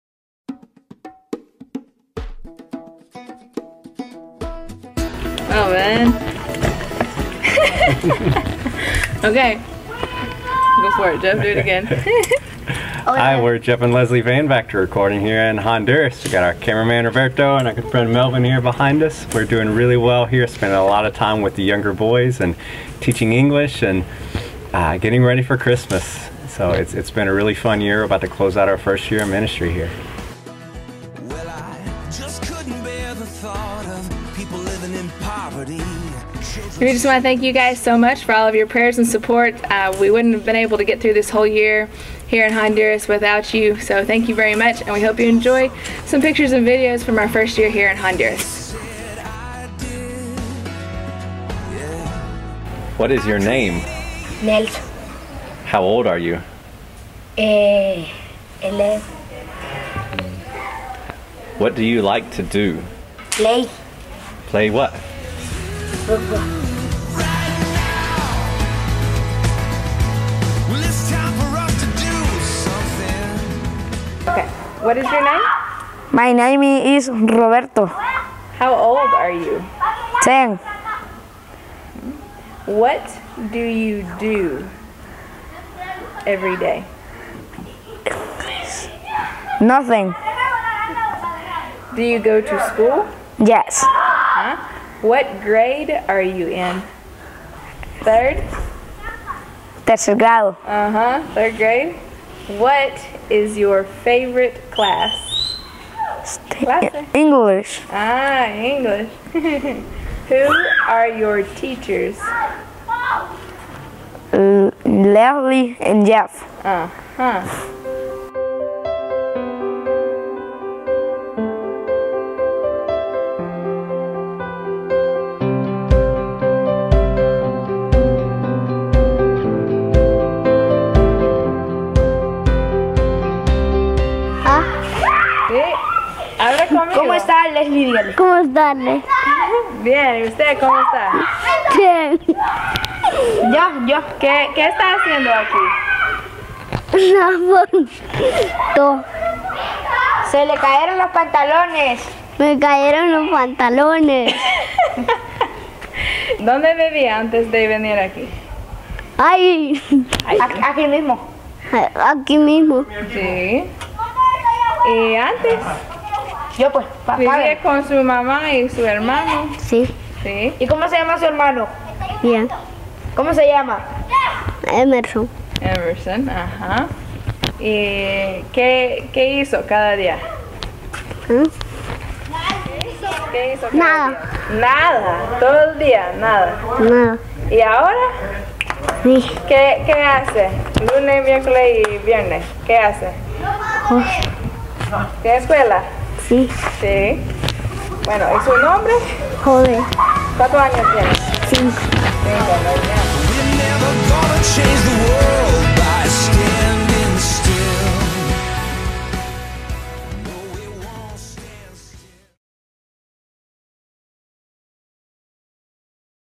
Honduras - Jeff & Leslie Vanvactor
This is not a bee video. My daughter and her husband are missionaries in Honduras and this is a short video of their first year on the mission field with Forgotten Children Ministries.